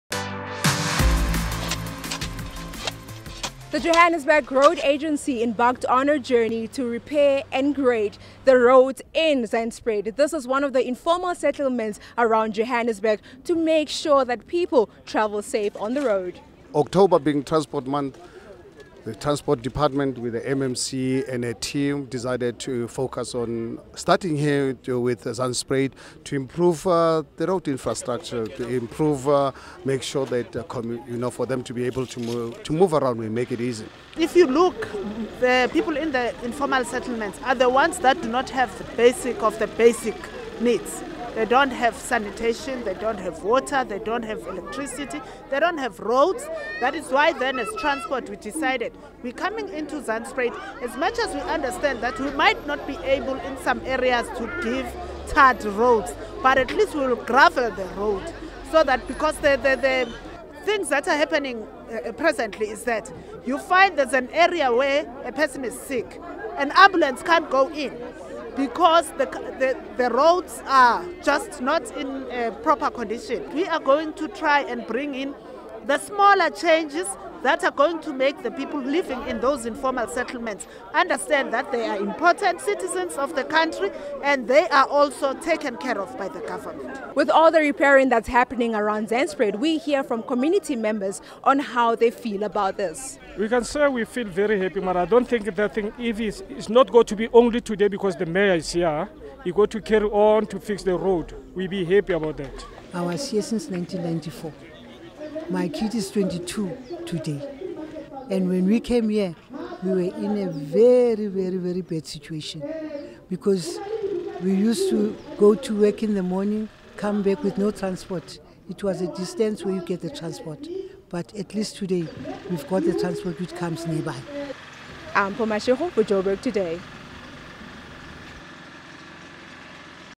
City News JRA Road Infrastracture
CITY NEWS - The Johannesburg Roads Agency Road(JRA) with the City of Joburg, led a clean-up drive in Zandspruit, improving the road infrastructure and more.